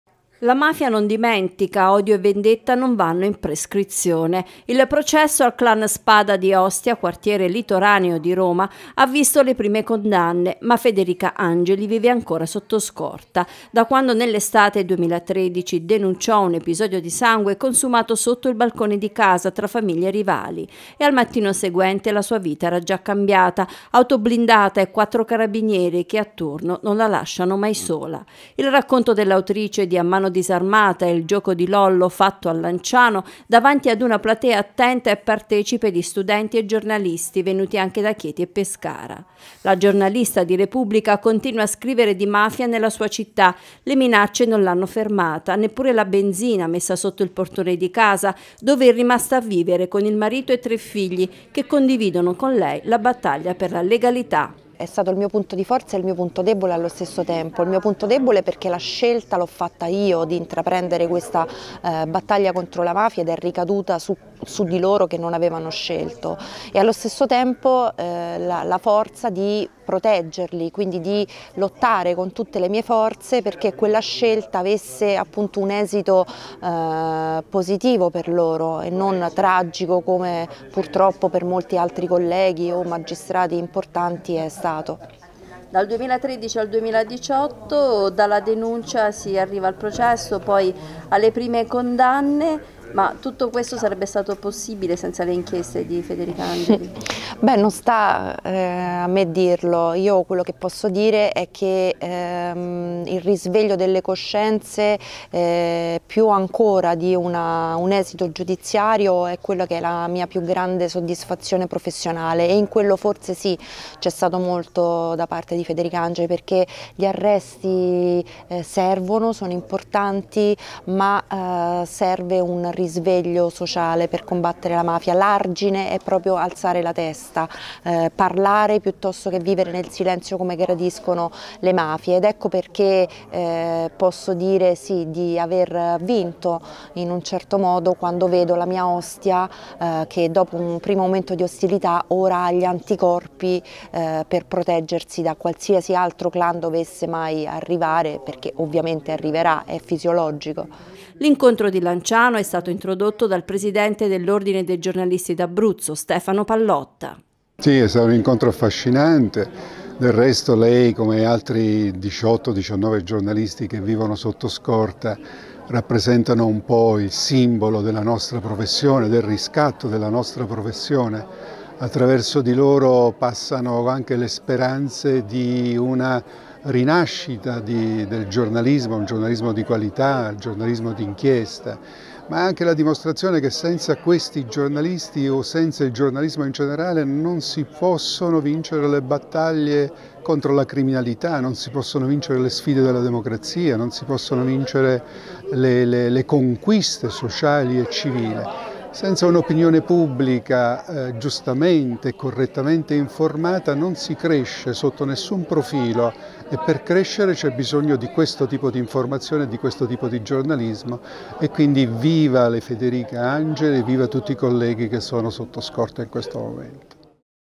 Federica Angeli incontra studenti e giornalisti a Lanciano, una vita blindata
La giornalista di Repubblica Federica Angeli incontra studenti e giornalisti a Lanciano, una vita blindata dopo le denunce e le inchieste sulla mafia di Ostia.